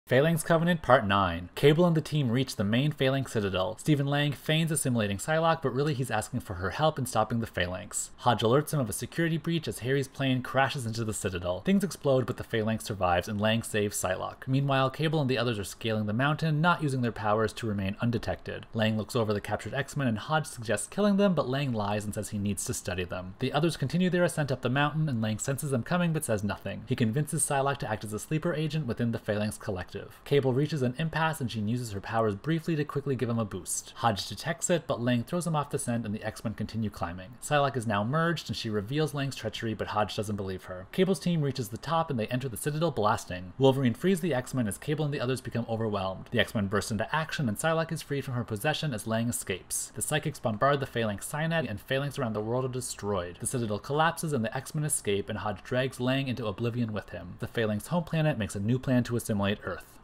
X-Men Phalanx Covenant (9 of 9) - Cable # 16 | Cyclops, Jean Grey, Wolverine, Psylocke, + MORE!
Cable’s team makes it to the Phalanx Citadel and needs to free the X-Men before the Phalanx takes total control of the Earth!

Phalanx Covenant (9 of 9) - Cable # 16 in 60 seconds!

Follow for more Great X-Mentations!

CABLE # 16

Featured Characters:
Cable
Cyclops
Phoenix (Jean Grey)
Wolverine
Psylocke
 Beast
Archangel
Bishop
Storm
Gambit
Iceman
Rogue

Antagonists:
The Phalanx
Stephen Lang
Cameron Hodge
Brick
Dymphna

Marvel Staff:
Editor-in-Chief - Tom DeFalco
Cover Artist - Steve Skroce / Mike Sellers
Writer - Larry Hama
Penciler - Steve Skroce
Inker -  Mike Sellers / Matt Banning
Colourist - Marie Javins
Letterer - Richard Starkings / Comicraft
Editor - Lisa Patrick / Bob Harras